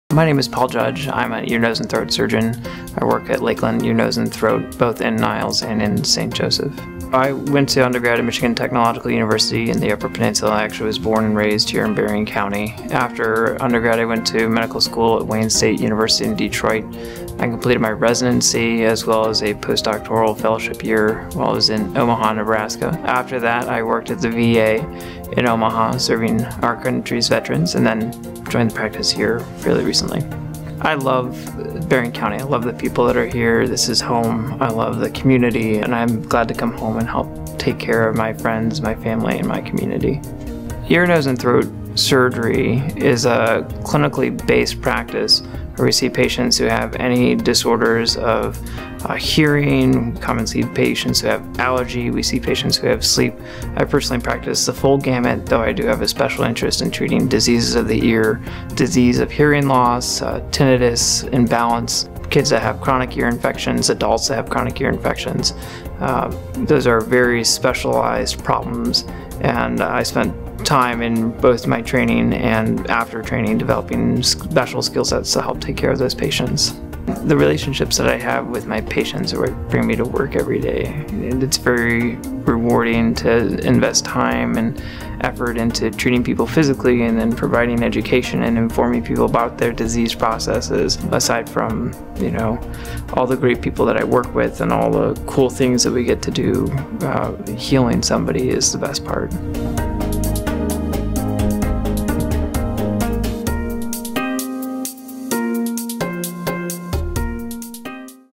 Physician Video Profile: Paul Judge, MD (Ear, Nose, & Throat)
Introducing ear, nose and throat specialist and otolaryngologist, Paul Judge, MD, who is practicing with Lakeland Ear, Nose and Throat.